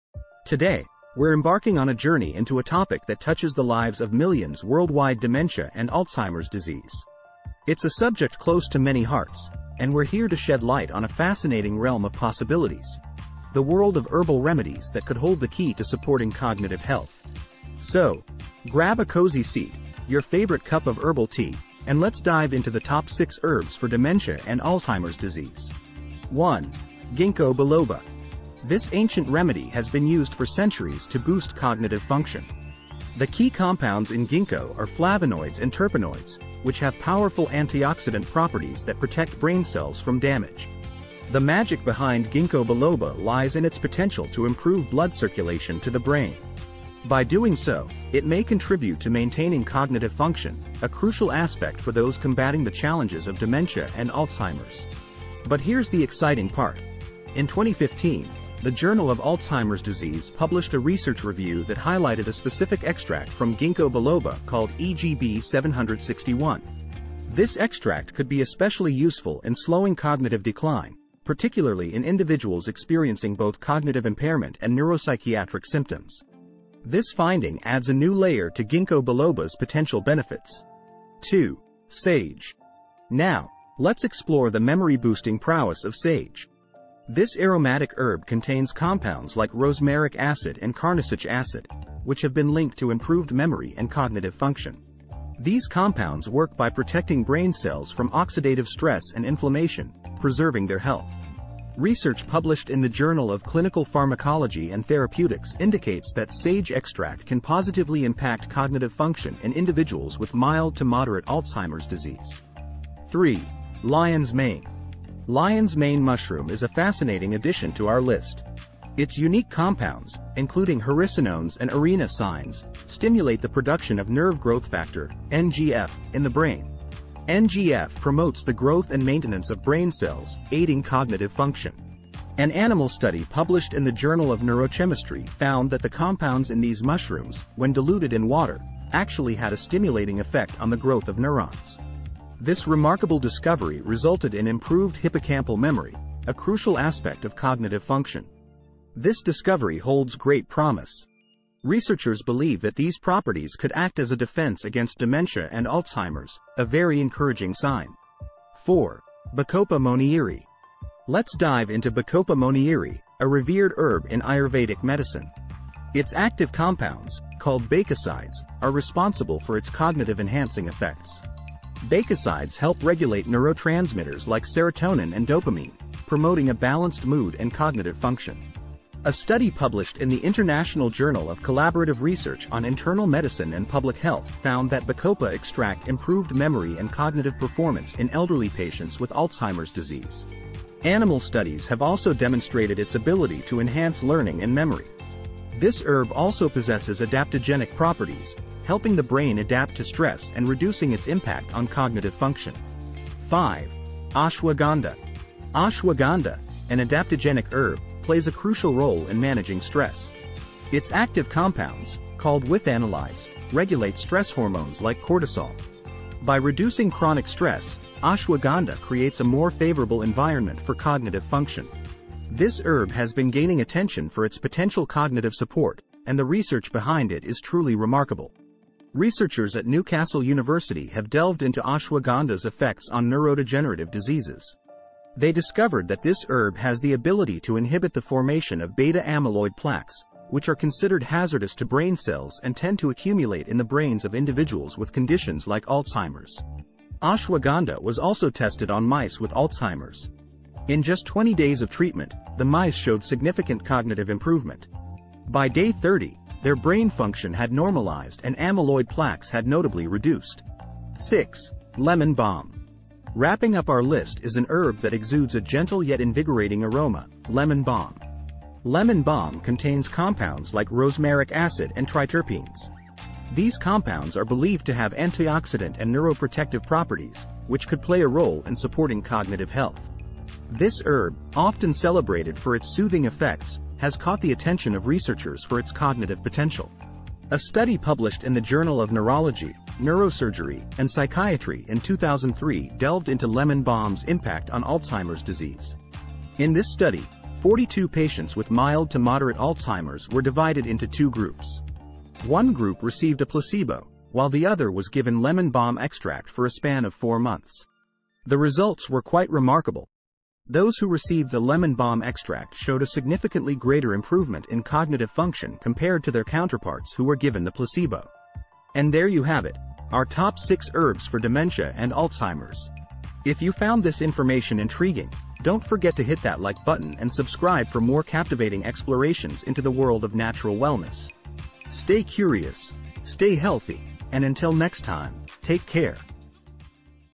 Top 6 Herbs for Dementia and Alzheimer's Disease (Backed By Science)
Herbs for Dementia and Alzheimer's Disease: Unveiling Nature's Potential 🌿 Discover the top 6 herbs that could hold the key to supporting cognitive health in our latest video.

From Gingko Biloba's brain-boosting effects to Lemon Balm's soothing cognitive benefits, we explore cutting-edge research and age-old wisdom. Learn how Bacopa Monnieri's cognitive prowess wowed researchers and how Ashwagandha's adaptogenic powers impressed scientists.

Dive into the world of Lion's Mane and its neuron-stimulating potential. Join us on this insightful journey as we uncover the science behind these herbs and their promise in the fight against cognitive decline. Stay informed, stay empowered.